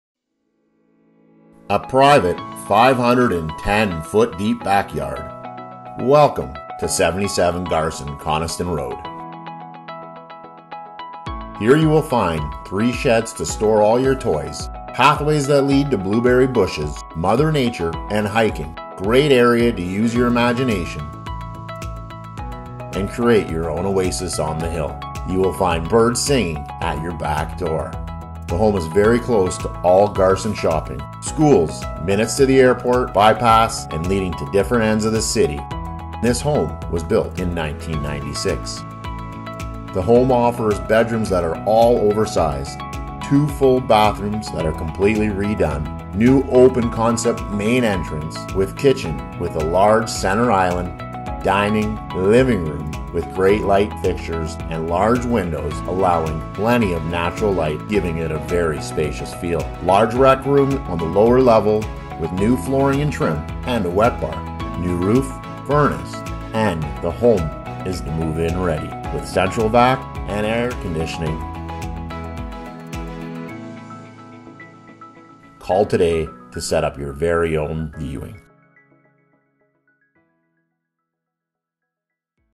Beautiful Home with Massive 510 ft. Backyard - Garson Coniston Rd
A private 510 deep foot backyard
Welcome to 77 Garson Coniston road.

Here you will find 3 sheds to store all your toys, pathways that lead to blueberry bushes, mother nature and hiking. A great area to use your imagination and create your own oasis on the hill.

You will find birds singing at your backdoor. The home is very close to all Garson shopping, schools, mini-city airports, bypass, and leading to different ends of the city.

This home was built in 1996. The home offers bedrooms that are all oversized and two full bathrooms that are completely redone. New open concept main entrance, a kitchen with a large centre island, a dining, living room with great light fixtures, and large windows allowing plenty of natural light to give a very spacious feel. Large rec room on the lower level with new flooring and wet bar, new roof, furnace

And the home is move-in ready with central vac and air conditioning.

Call today to set up your very own viewing.